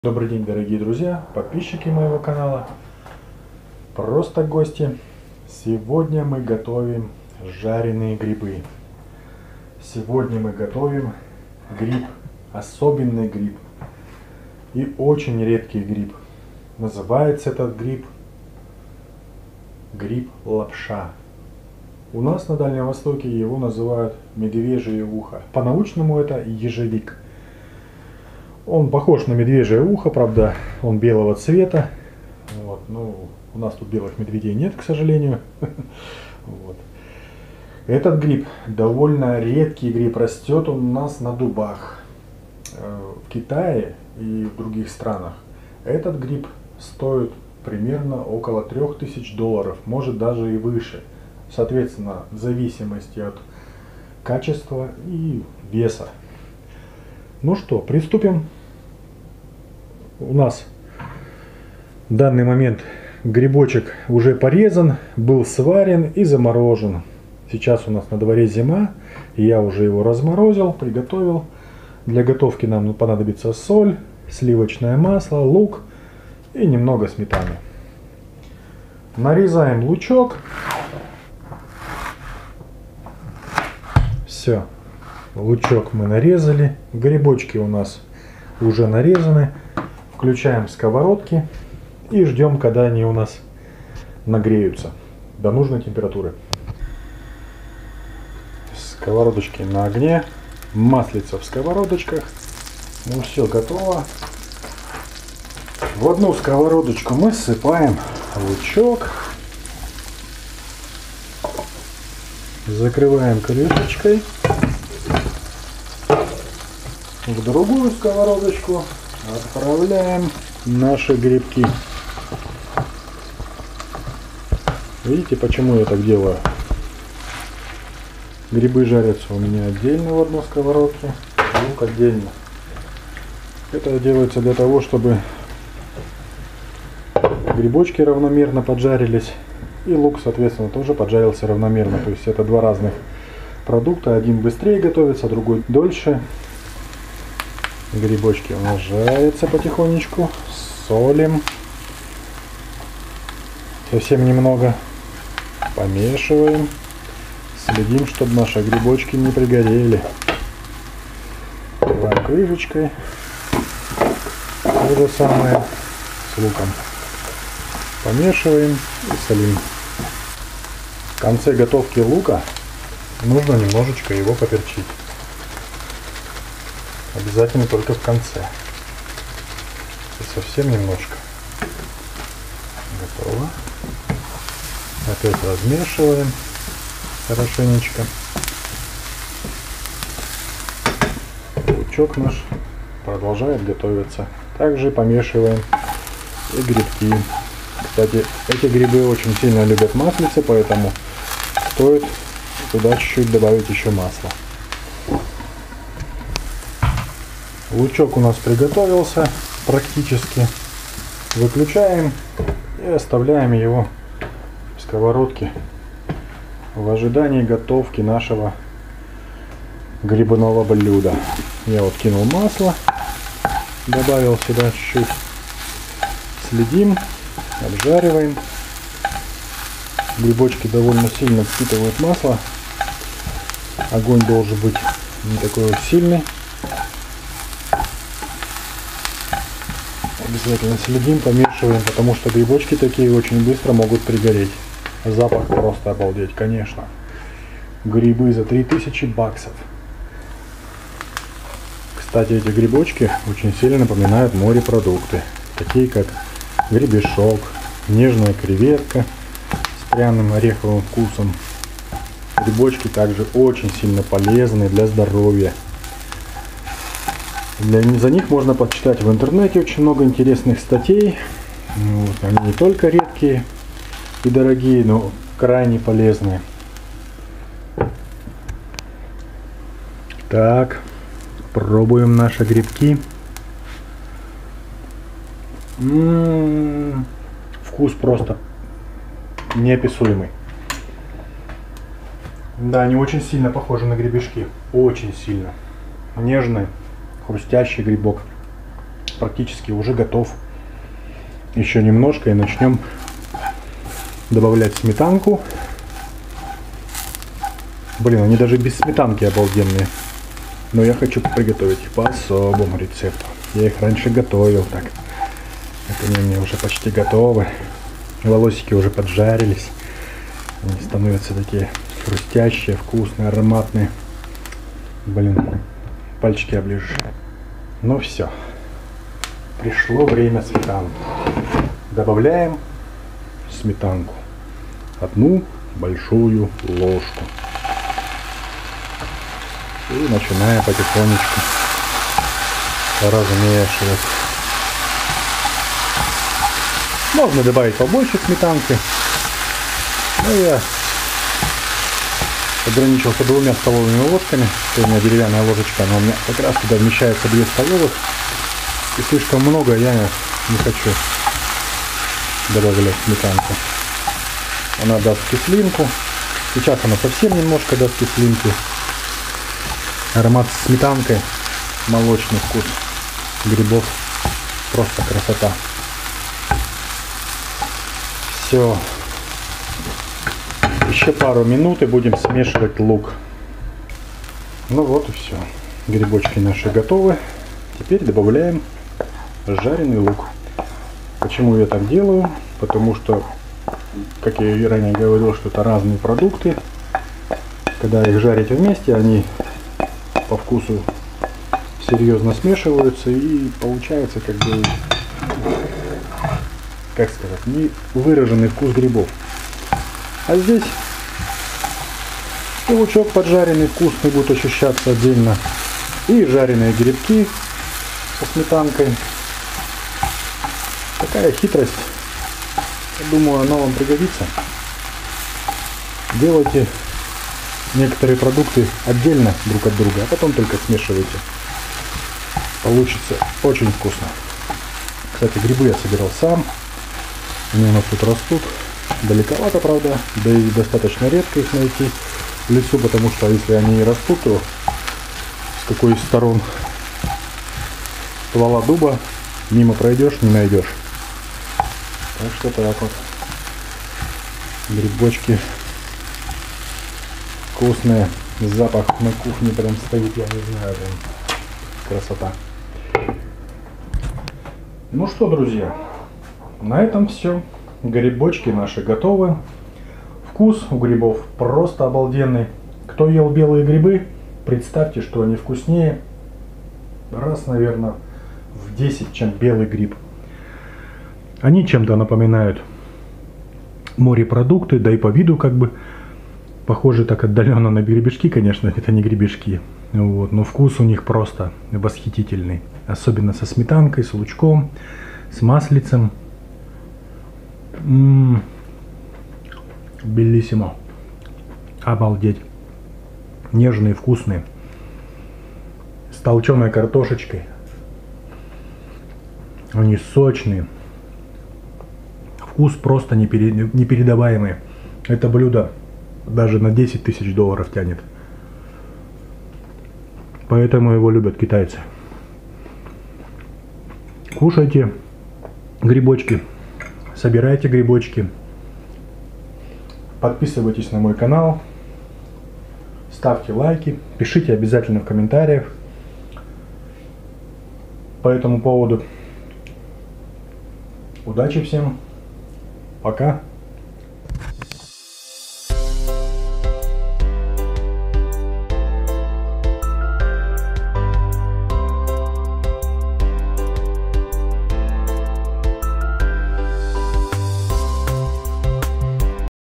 Гриб за 3000$. Медвежье УХО! Жареный гриб лапша. Ежовик.
Добрый день дорогие друзья. Сегодня мы готовим редкий полезный и дорогой гриб. Ежовик, гриб лапша или медвежье ухо. Так его называют. Очень вкусный полезный и красивый гриб. Гриб, который стоит в Китае 3000$ долларов и более. В зависимости от размера и качества, а также, чтобы он был дикий. Гриб имеет неповторимый вкус! Вкус морепродуктов с нотками ореха. Больше по вкусу к гребешкам.
Мы готовим его со сметаной и луком.
Рецепт:
1 кг гриба
100гр масла
Луковица
Сметана 60 гр
Соль и перец черный молотый по вкусу.
Грибы белые сбор

Этим вы поддерживаете нас! И мы в свою очередь будем стараться делать видео обзоры еще интереснее!!! Спасибо Вам!!!